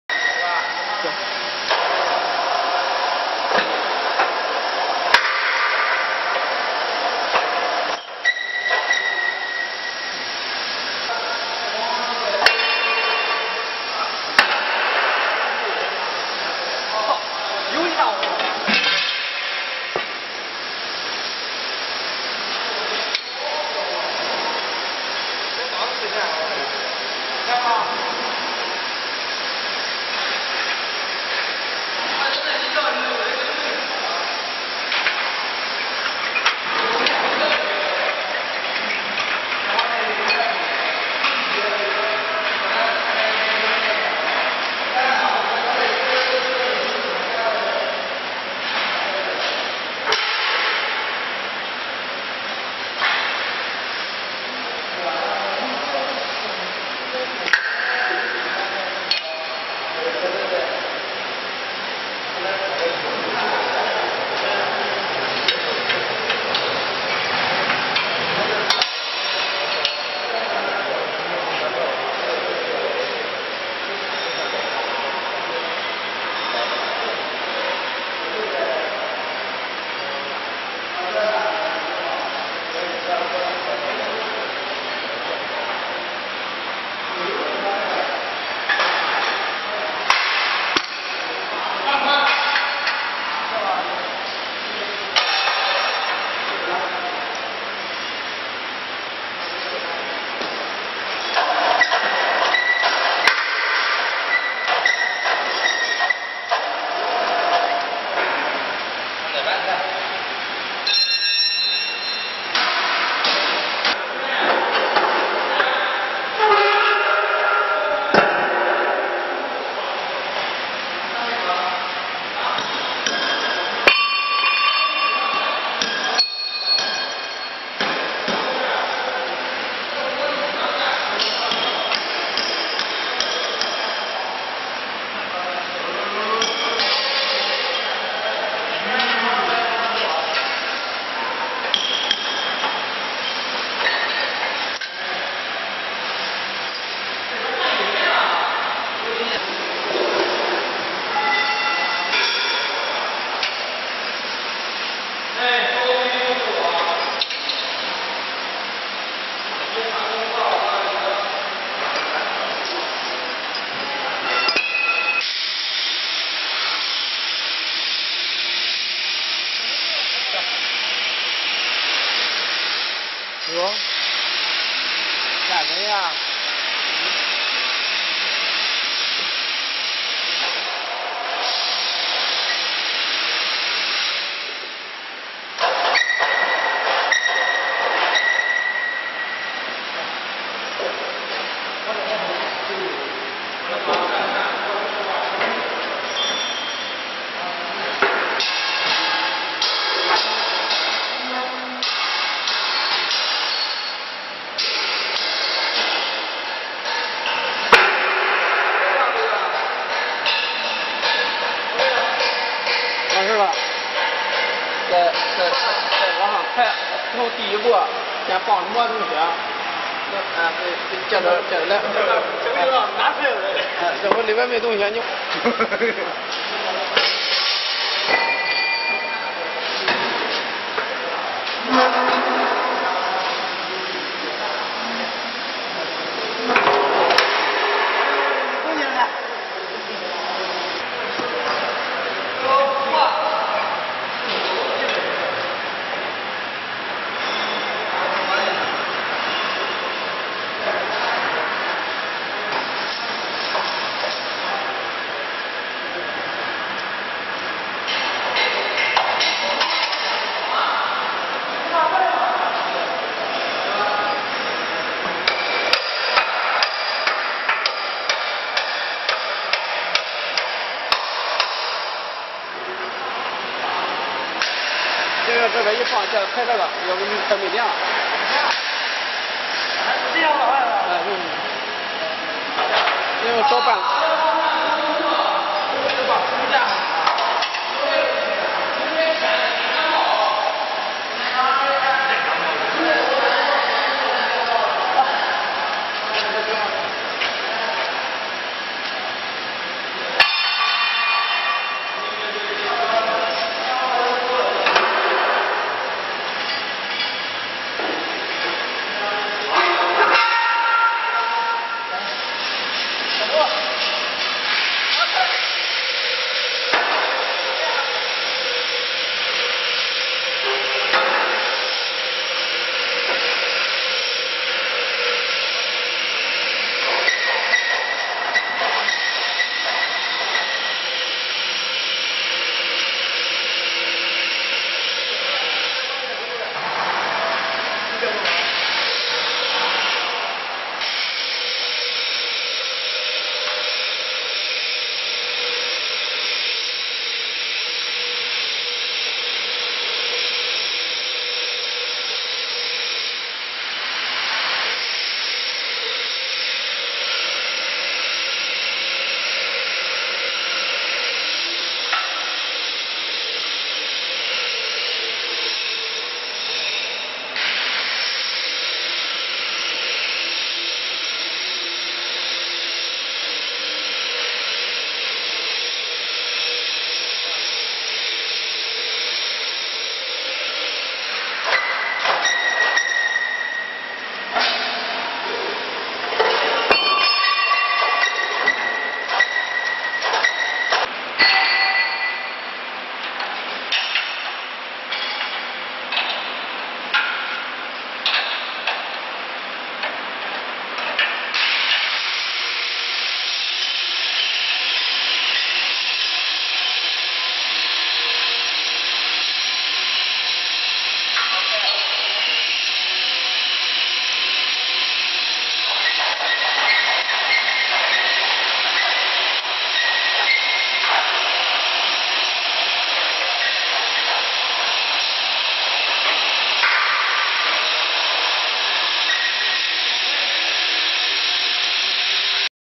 Change The Mould Of QTJ4-40 Brick Making Machine From Pompom China
Spokesperson:Pompom